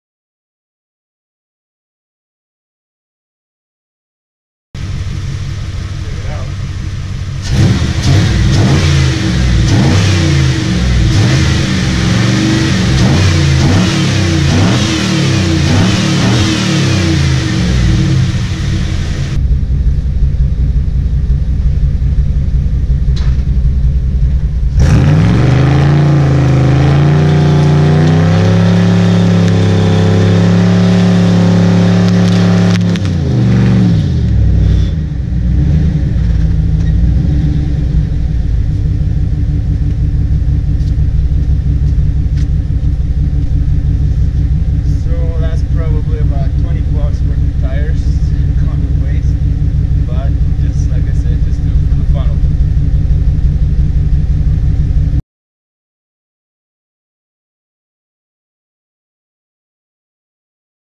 Doing Burnout with the LT1 Trans Am
Earlier during the day I uploaded a Video doing a follow-up on replacing a bad Fuel Injector on the LT1 Trans Am, during that Video I included a footage doing throttle response test and a quick Burn-out.
Since that was my favorite part of the Video and I don't want to keep rewinding the entire Video to hear the Sound of the Engine as I rev it up and the cool look of smoking the rear Tires doing the Burnout I made this version that only has that part.
The camera doesn't do justice as far as the Sound of the Engine because it sounds way better in real life but adjusting the Bass sounds closer to reality.
I am not sure how many other people like the Sound of an LT1 revving up and Burn-outs but for the ones who like the same thing I do I hope you like this Version of the Video.
As the Tires go up in Smoke I can see my local Tire dealer where I bought these tires hearing the $$$ dropping in just like winning the jack pot in the Slot Machine at the Casino. ( You are Welcome ).